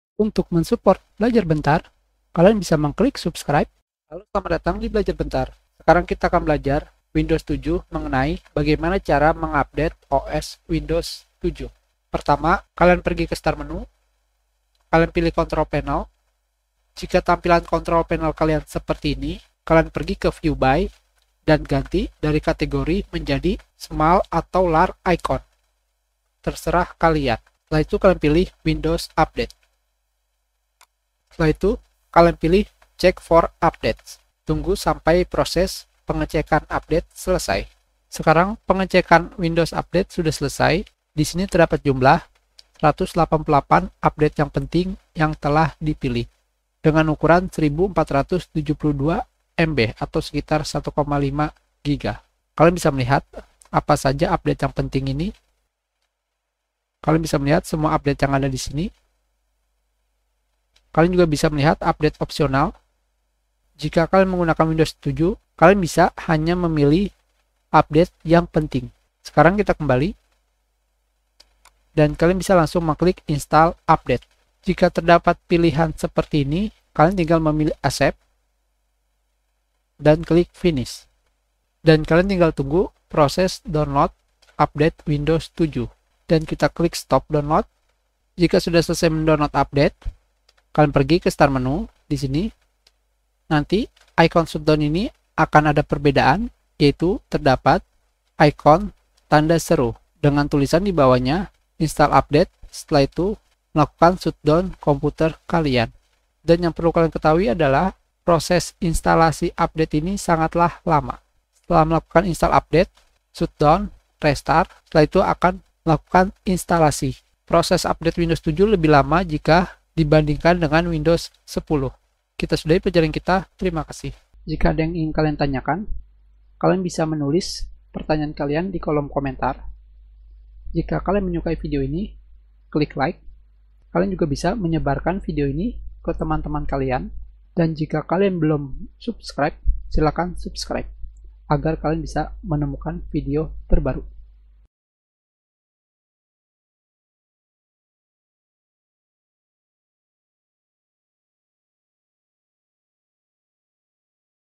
Belajar Windows 7 Cara Update ke Versi Terbaru. Indonesia
Sekarang kita akan belajar Windows 7 mengenai cara update ke versi terbaru.

Update ini bisa kalian gunakan pada windows 7 versi starter, home basic, home premium, professional, enterprise, ultimate

!Windows 7 sudah tidak disupport lagi (EOL)